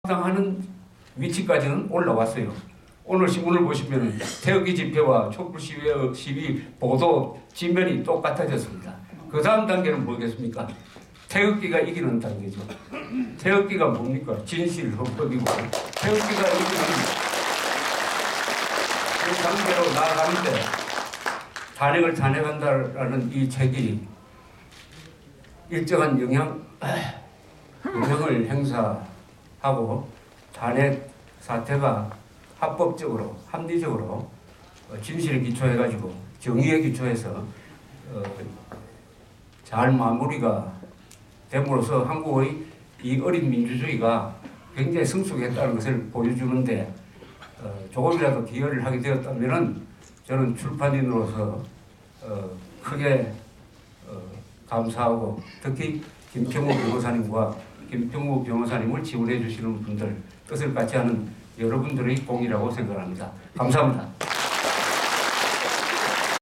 서평. 조갑제 조갑제닷컴 대표: "이번 탄핵 사태는 언론의 난"(2)
2월 13일 한국 프레스센터 20층 국제회의장에서 열린 '탄핵을 탄핵한다'의 저자 김평우 출판기념 강연회